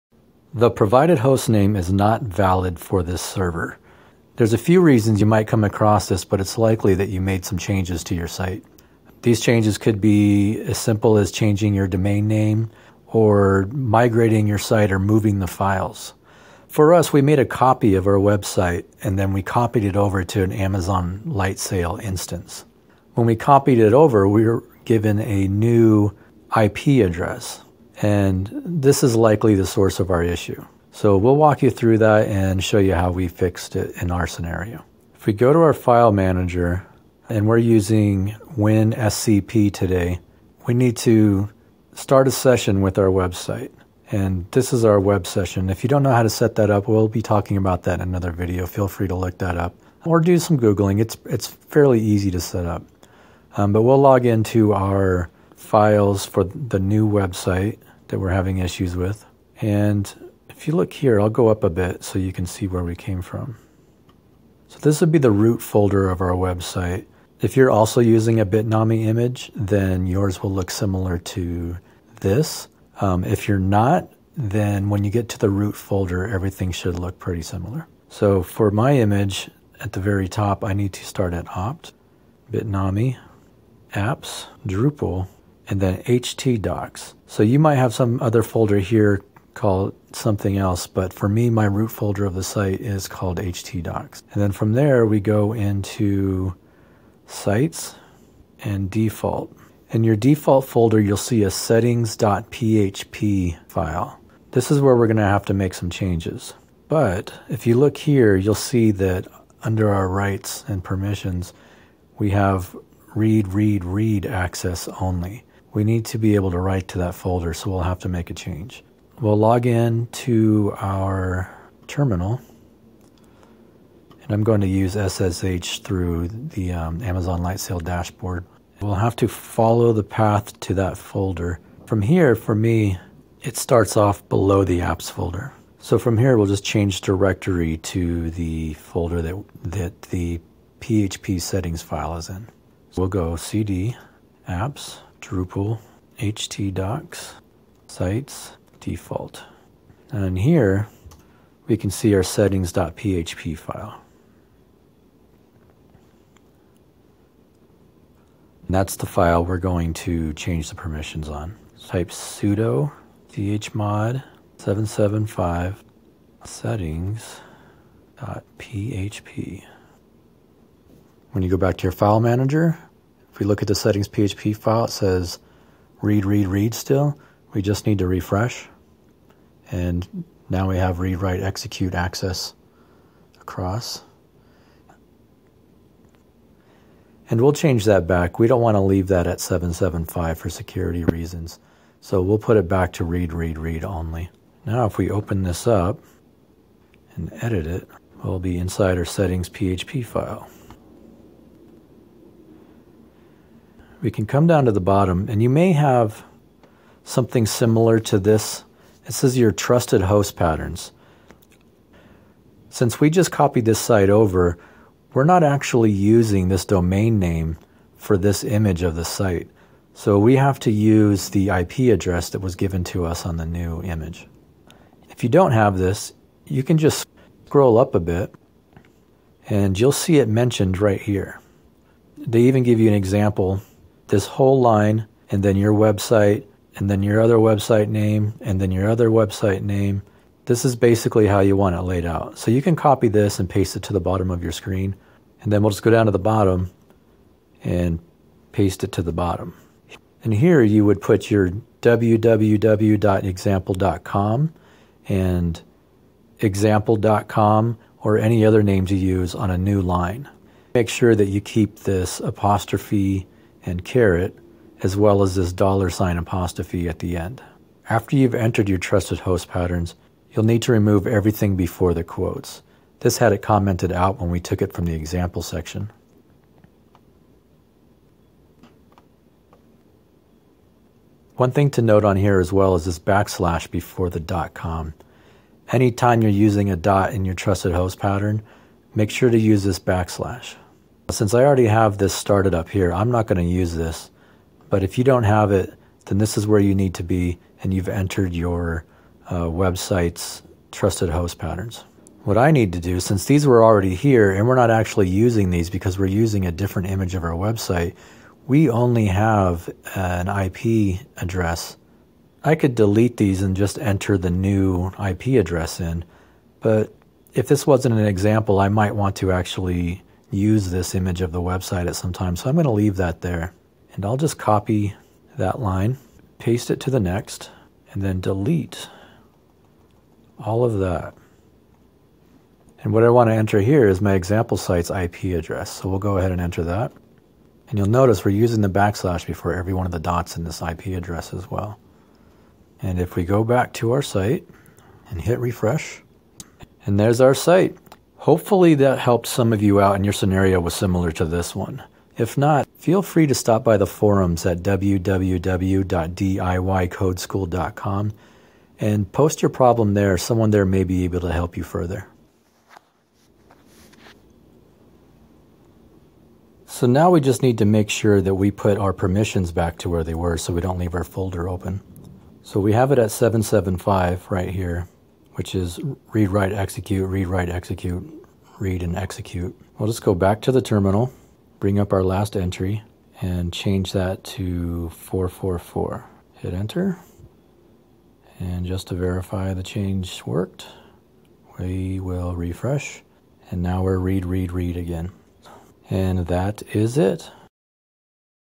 Drupal 8- HOST NAME NOT VALID - Trusted host patterns
If you come across "The provided host name is not valid for this server" this video may help show an easy action to correct it with file locations explained.